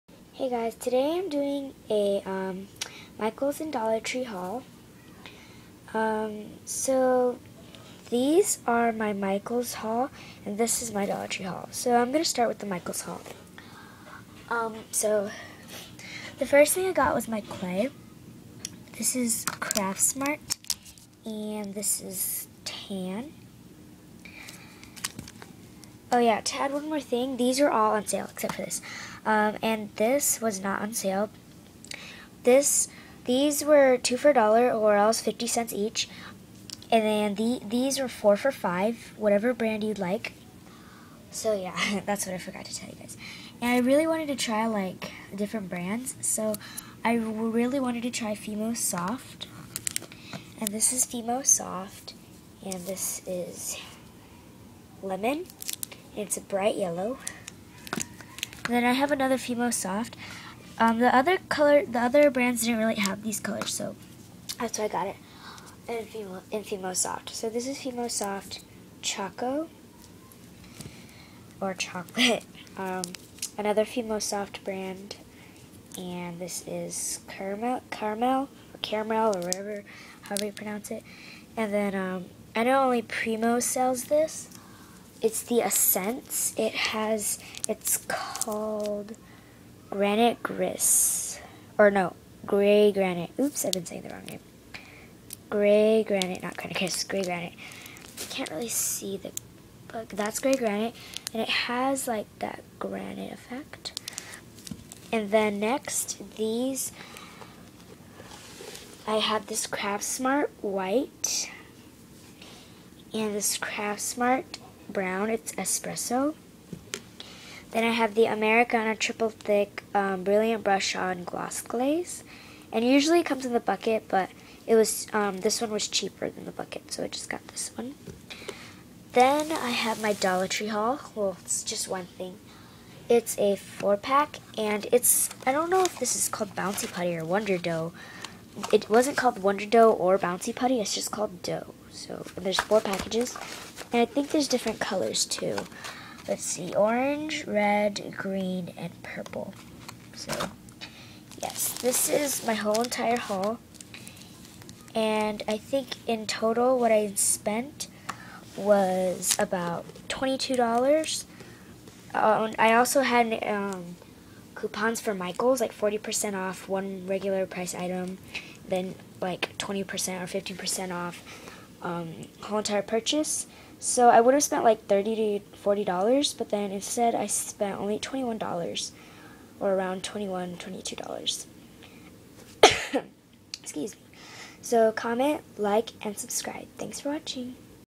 Michaels And Dollar Tree Haul
Craftsmart polymer clay- tab
Fimo soft- lemon
Fimo soft- choco
Fimo soft- caramel
Craftsmart- white
Craftsmart- espresso
Dough- orange purple red blue
 Polymer clay 4 for $5
Acrylic paint 2 for $1 craftsmart